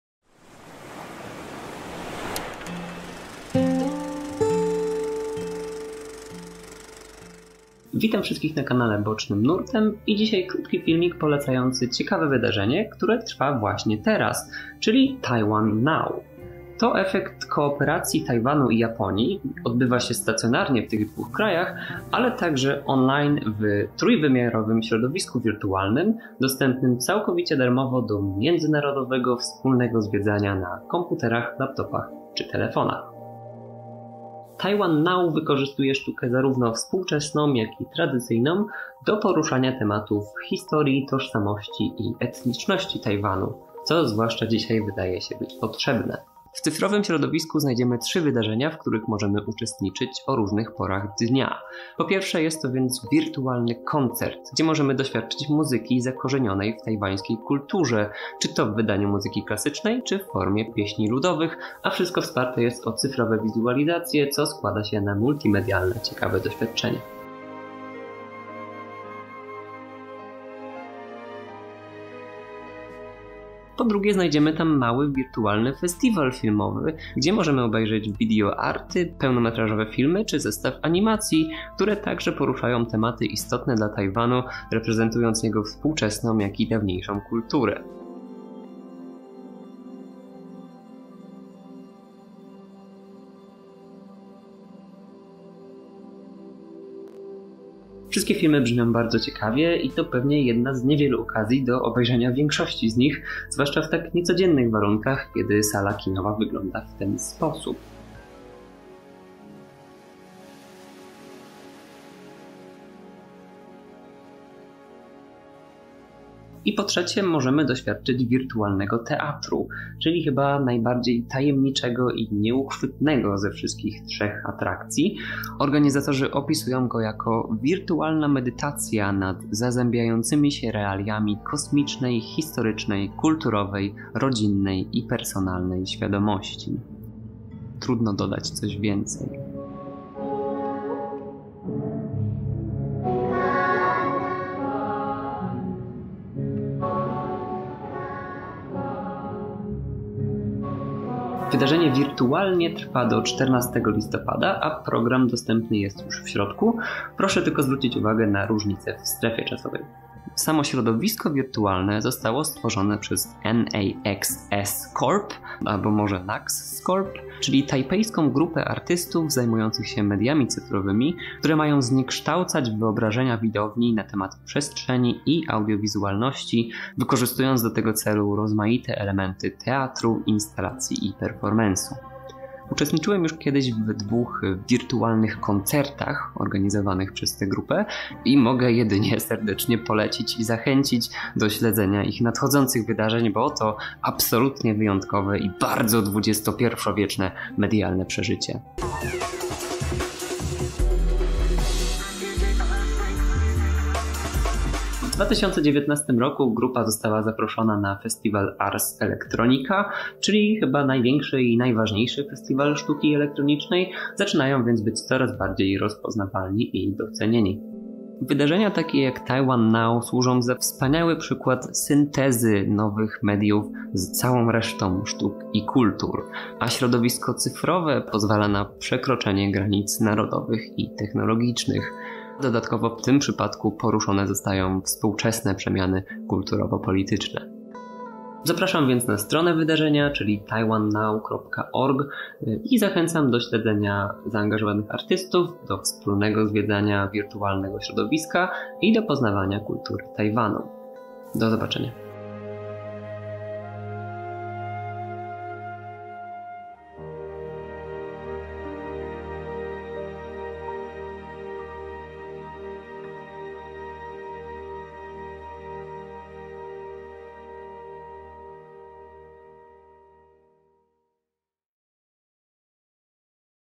Taiwan NOW – Taiwanese festival of contemporary art
Taiwan NOW is a Taiwanese-Japanese collaboration, bringing together artists from various backgrounds, presenting the contemporary culture and realities of Taiwan. The event is enriched by a virtual space, created by NAXS corp., Which allows free participation in the festival online all over the world.
The festival runs from October to December 2021, and the virtual version from October 30 to November 14.

- NAXS corp website. (warning of flashing lights)

Part of the event is a small festival / film review, presenting both Taiwanese short films (made in the last 3 years), as well as animations and video art. Taiwan cinema is still not very popular and can be difficult to access anywhere other than individual film festivals, so this is a great opportunity!